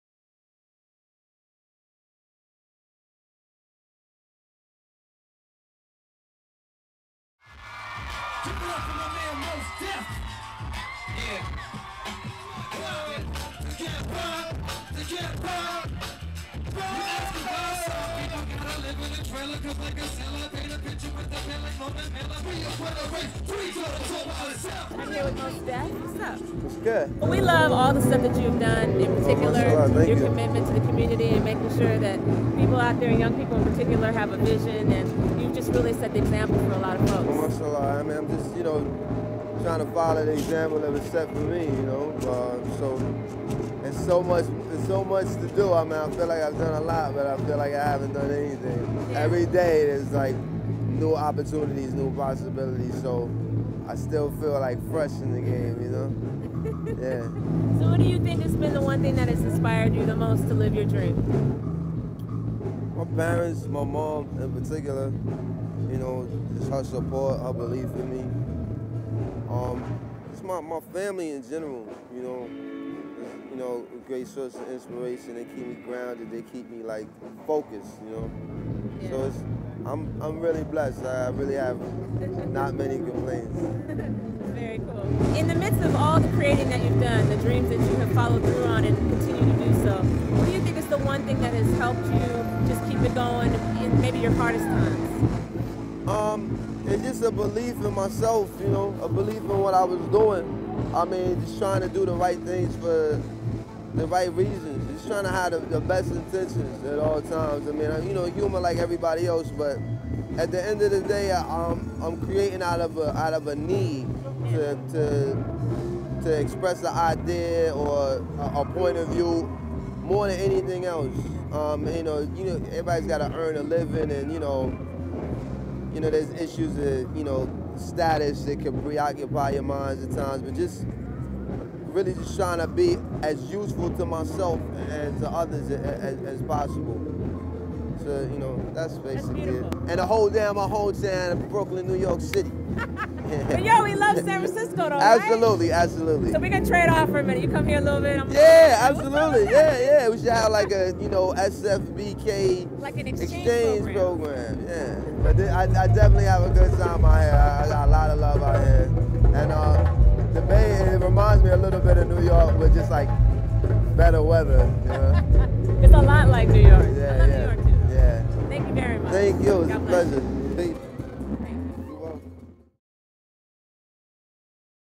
Mos Def
dayswithzahrah interviews Mos Def